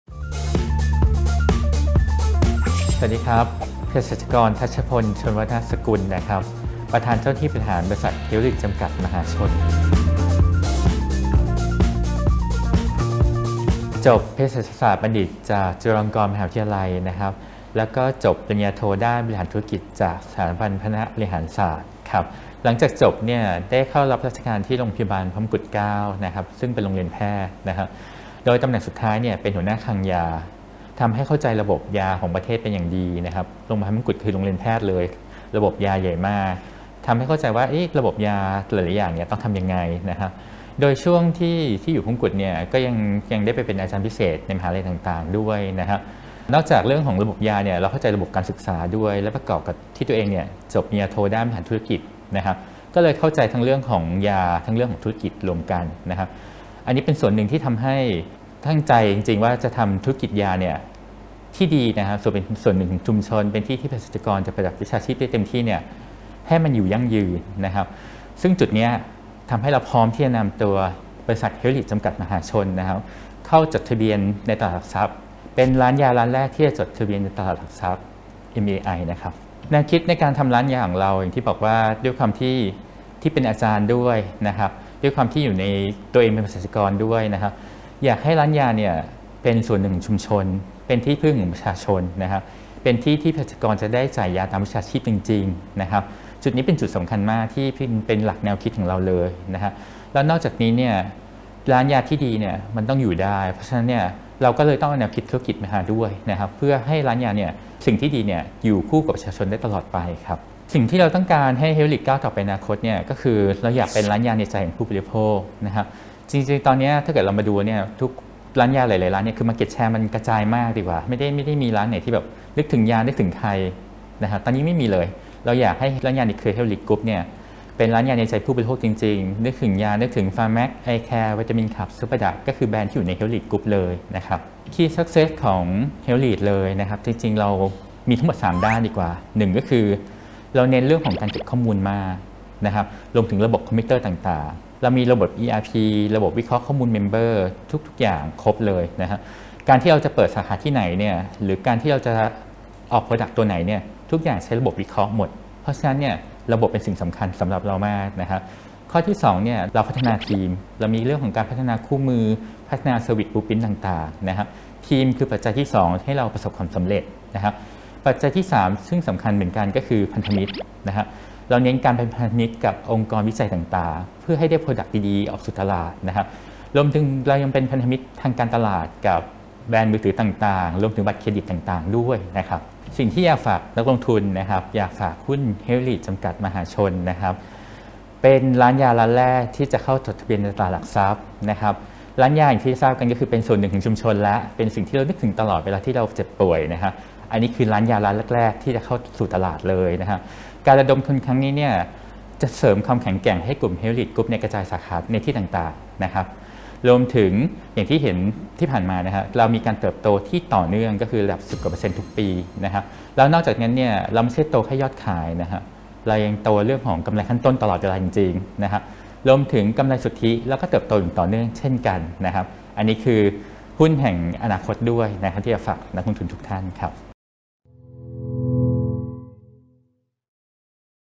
"เฮลท์ลีด" ร้านขายยารายแรก ในตลาดหลักทรัพย์ฯ
สัมภาษณ์พิเศษ ภก.ธัชพล ชลวัฒนสกุล ประธานเจ้าหน้าที่บริหาร บริษัท เฮลท์ลีด จำกัด (มหาชน) หรือ HL

ติดตามทันหุ้นได้หลากหลายช่องทาง